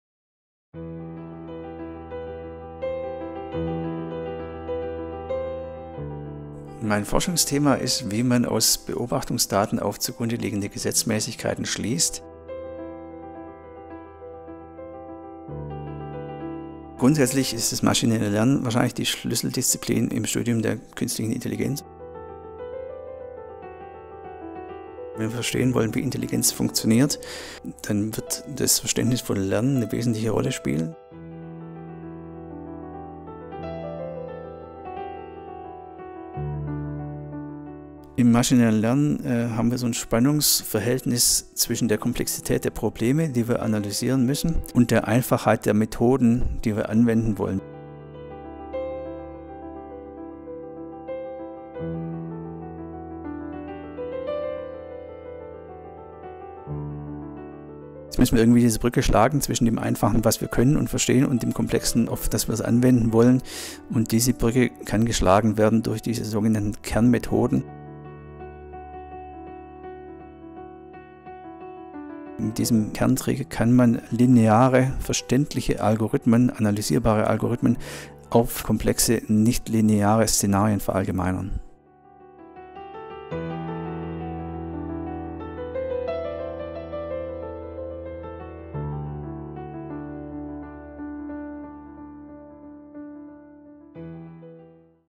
Prof. Dr. Bernhard Schölkopf im Porträt
Filmporträt anlässlich des Gottfried Wilhelm Leibniz-Preises der Deutschen Forschungsgemeinschaft 2018 - mehr